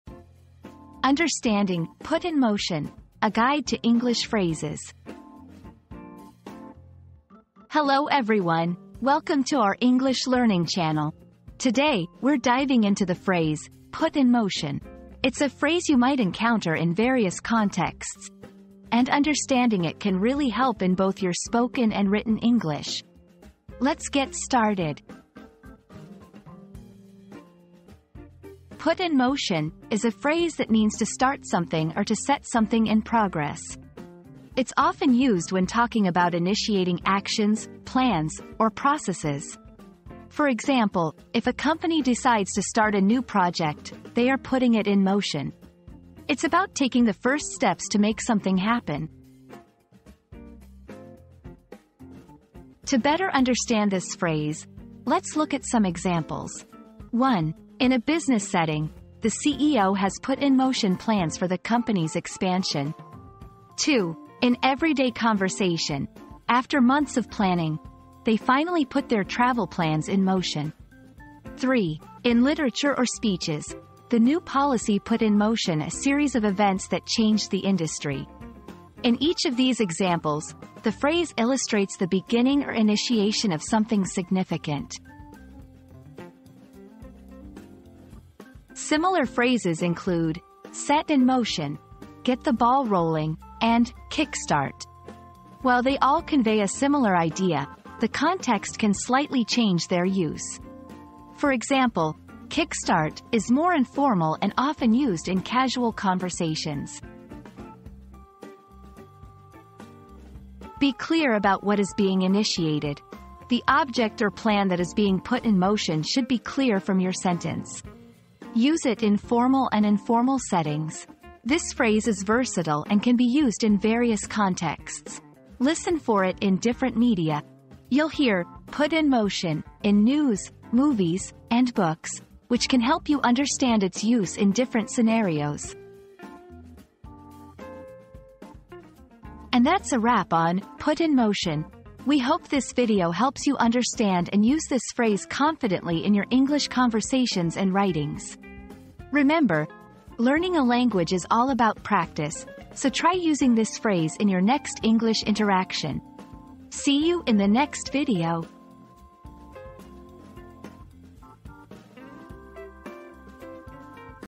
Understanding "Put in Motion": A Guide to English Phrases
00:00 • Introduction - Understanding "Put in Motion": A Guide to English Phrases
00:28 • The Meaning of "Put in Motion"
00:52 • Examples in Context
01:28 • Variations of the Phrase
01:50 • Tips for Using "Put in Motion"